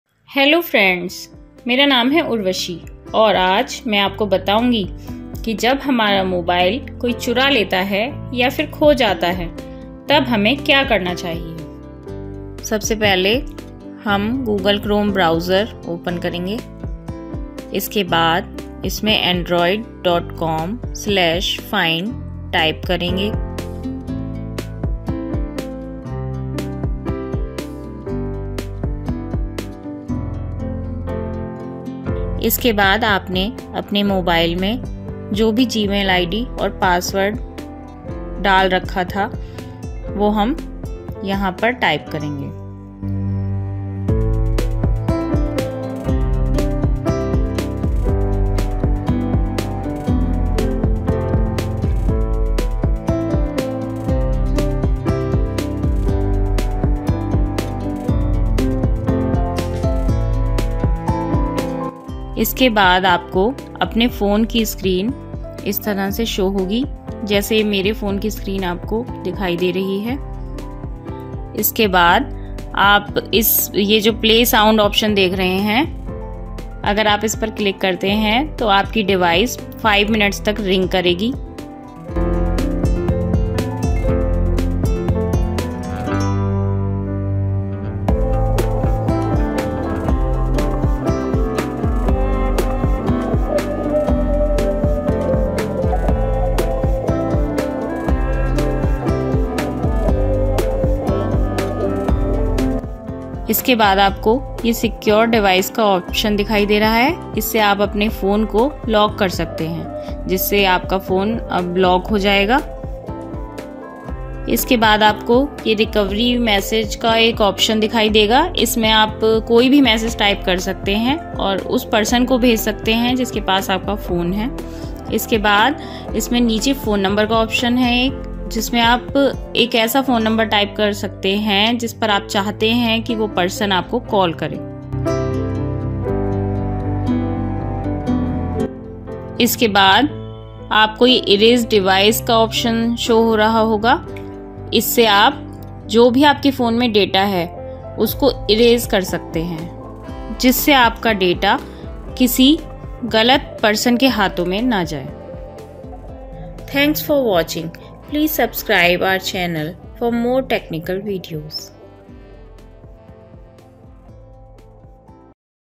How to Erase Data from Lost Phone. | Technical with Urvashi
How to Erase Data from Lost Phone.
अपने चोरी हुए मोबाइल को remotely लॉक कैसे करूँ  |
फ़ोन चोरी हो गया ? अब क्या करूँ ?
अपने चोरी हुए मोबाइल को remotely Ring/call कैसे करूँ
Technical with Urvashi